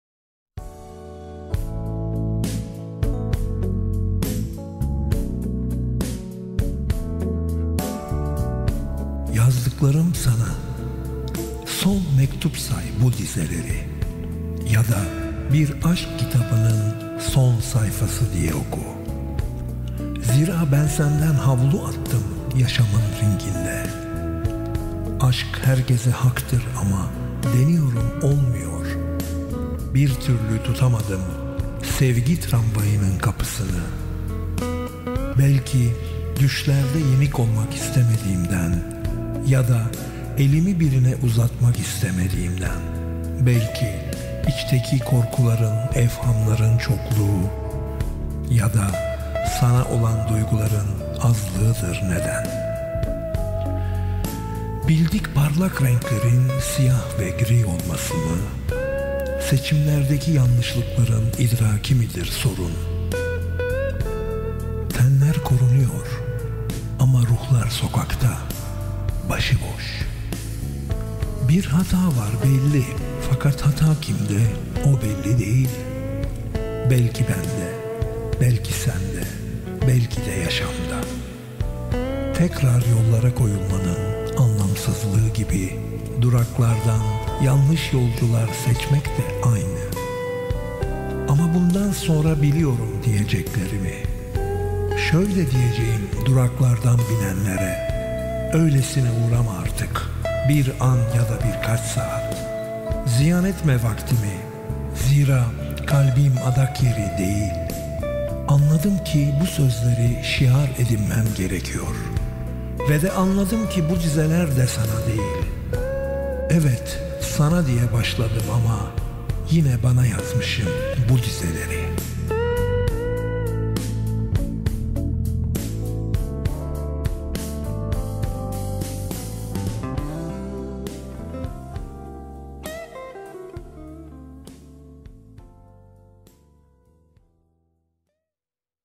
ŞİİR. KENDİME DİZELER (BOJİDAR ÇİPOF)
Yazdıklarım sana, son mektup say bu dizeleri

Ya da bir aşk kitabının son sayfası diye oku

Zira ben senden havlu attım yaşamın ringinde

Aşk herkese haktır ama deniyorum olmuyor

Bir türlü tutamadım sevgi tramvayının kapısını

Belki düşlerde yenik olmak istemediğimden,

Ya da elimi birine uzatmak istemediğimden,

Belki içteki korkuların evhamların çokluğu,

Ya da sana olan duyguların azlığıdır neden

Bildik parlak renklerin siyah ve gri olması mı?

Seçimlerdeki yanlışlıkların idraki midir sorun?

Tenler korunuyor ama ruhlar sokakta başıboş

Bir hata var belli fakat hata kimde o belli değil

Belki bende, belki sende, belki de yaşamda

Tekrar yollara koyulmanın anlamsızlığı gibi,

Duraklardan yanlış yolcular seçmek de aynı

Ama bundan sonra biliyorum diyeceklerimi

Şöyle diyeceğim duraklardan binenlere:

“Öylesine uğrama artık bir an ya da birkaç saat

Ziyan etme vaktimi zira kalbim adak yeri değil”

Anladım ki bu sözleri şiar edinmem gerekiyor

Ve de anladım ki bu dizeler de sana değil

Evet, sana diye başladım ama

Yine bana yazmışım bu dizeleri…

Bojidar Çipof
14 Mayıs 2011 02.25